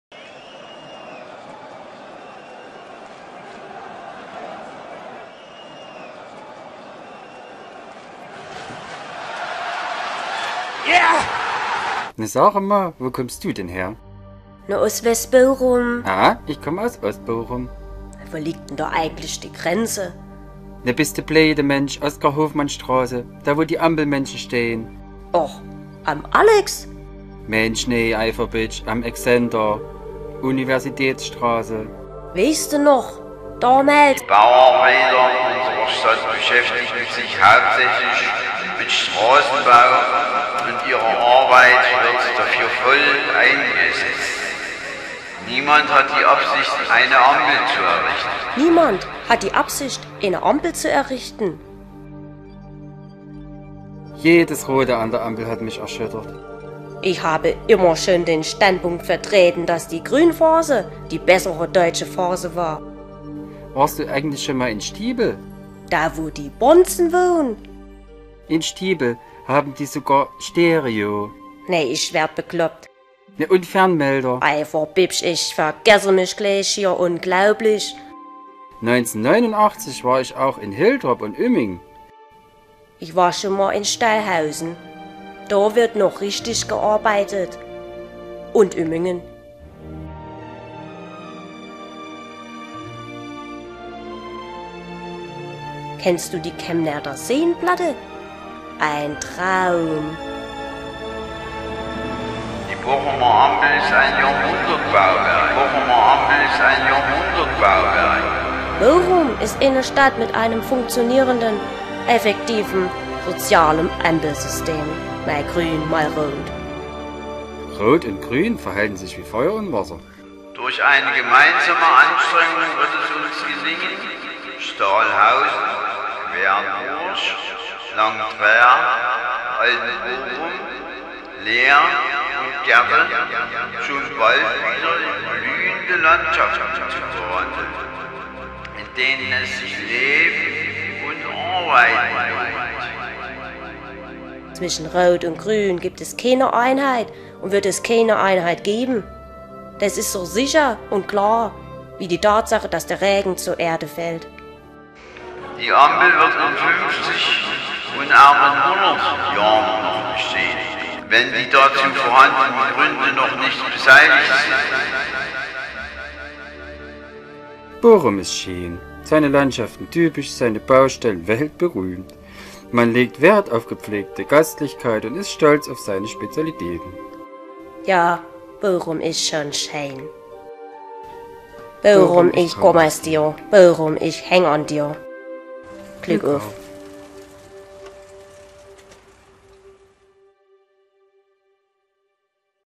Ampelmännchen in Ostbochum und Westbochum
Wo verläuft eigentlich die Grenze zwischen Ostbochum und Westbochum? Ein jetzt entdeckter historischer Stadtplan zeigt die alte Grenzmarkierung. Auch belegt er nun zweifelsfrei: Die Bauarbeiter unserer Stadt beschäftigen sich hauptsächlich mit Straßenbau und ihre Arbeitskraft wird voll dafür eingesetzt. Und nach Akteneinsicht steht nun fest: Die Bochumer Ampel ist ein Jahrhundertbauwerk.

Ostbochum. Westbochum. Heimat